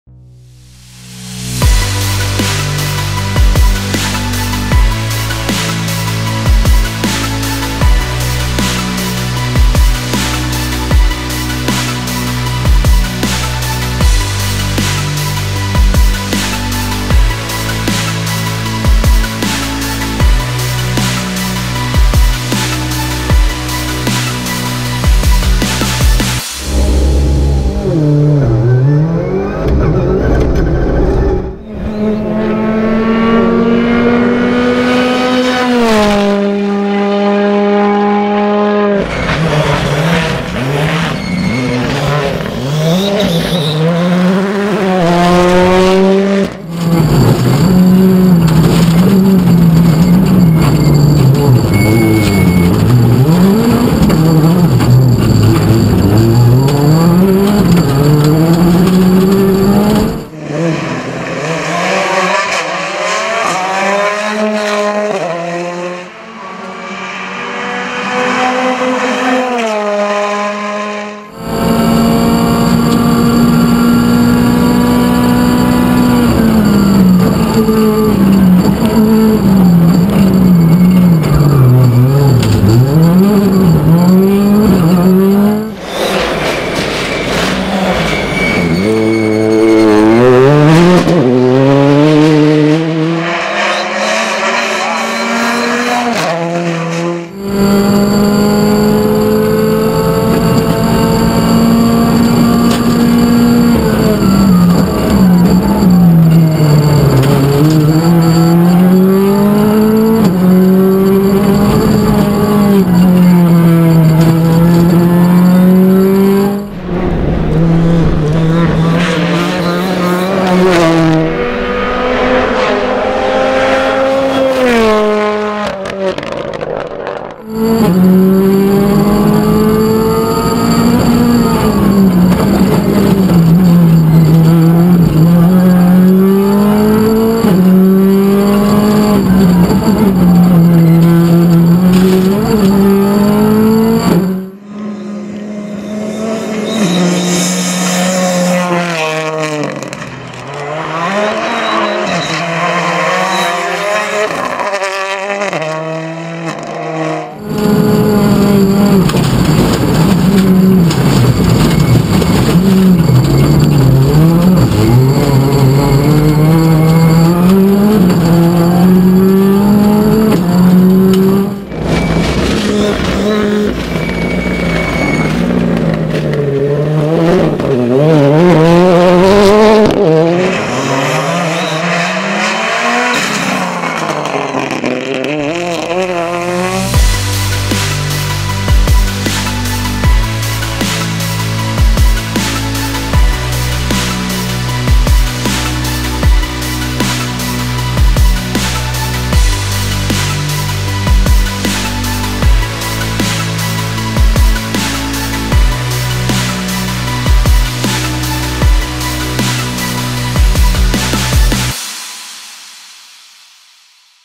Sezoensrally 2019 | Ford Fiesta R5 Stephen Wright & Eddy Smeets
Sezoensrally 2019 Stephen Wright co-driven by Eddy Smeets in the Ford Fiesta R5.